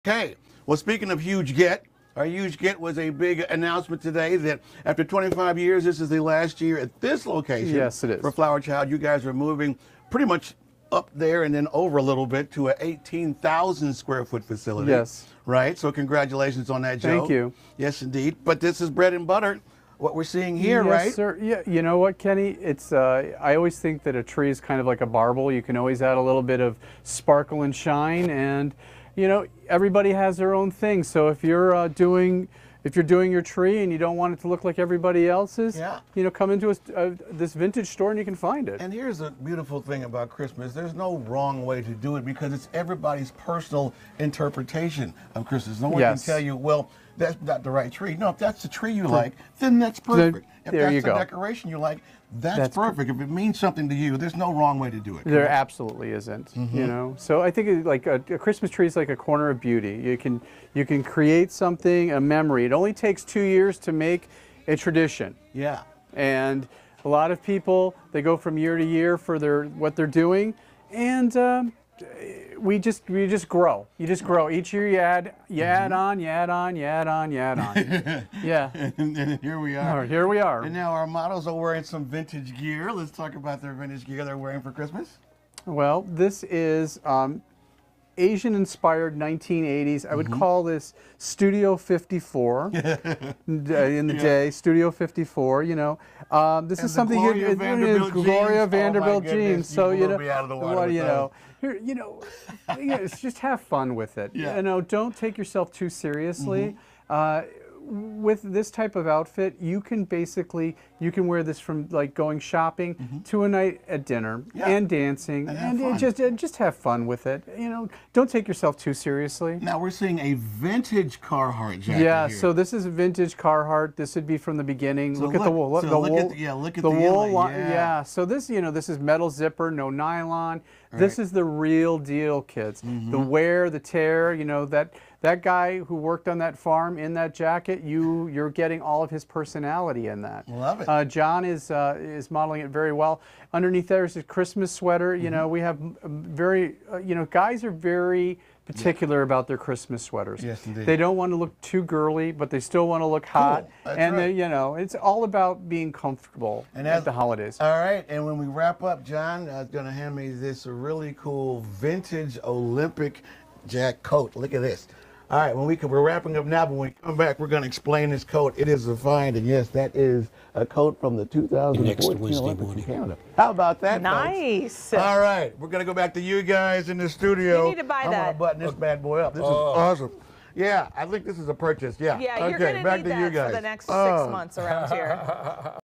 Flower Child gives Kenny all kinds of 'vintage' ideas for Christmas
Flower Child Vintage Store collects vintage items all year long so holiday shoppers can have a huge selection of home decor and Christmas gifts to choose from this time of year. Fox 8's Kenny Crumpton has an annual tradition of visiting the store at the beginning of the holiday shopping season to share a sneak peek of what's on the shelves. Flower Child is passionate about saving any 'vintage' item from landing in a landfill and is one of Northeast Ohio's most unique retail stores.